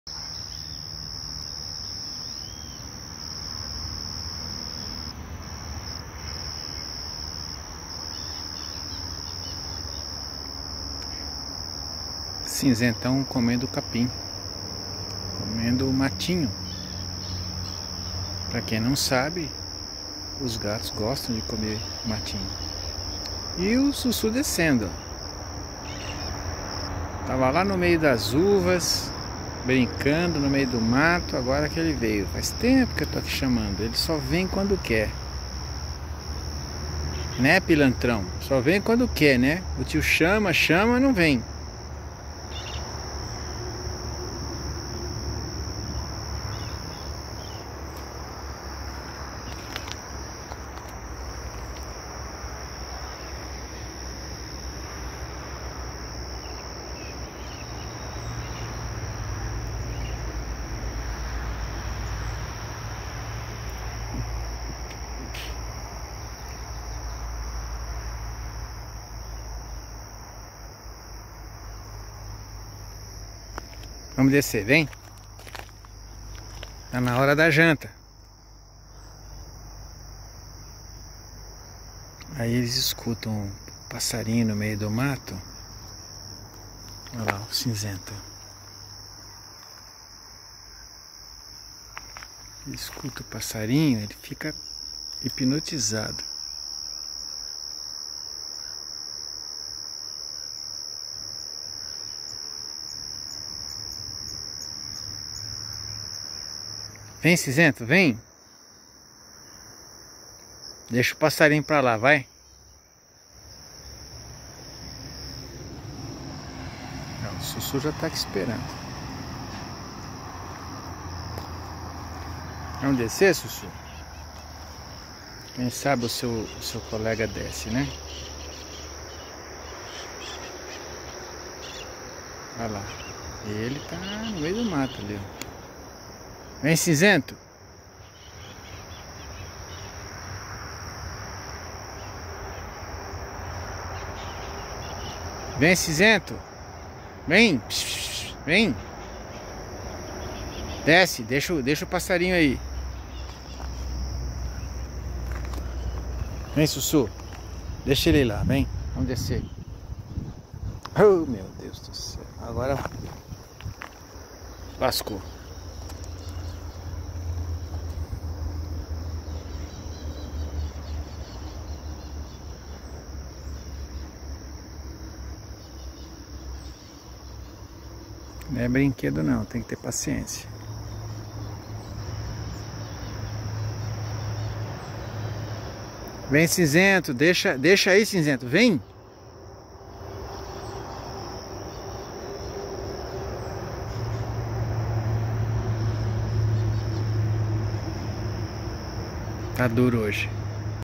Cinzento e Sussú passeando na chácara. Haja paciência...
Passear com os gatinhos exige paciência.
Quem quer cuidar deles deve se encher de muita calma, porque eles só fazem o que querem.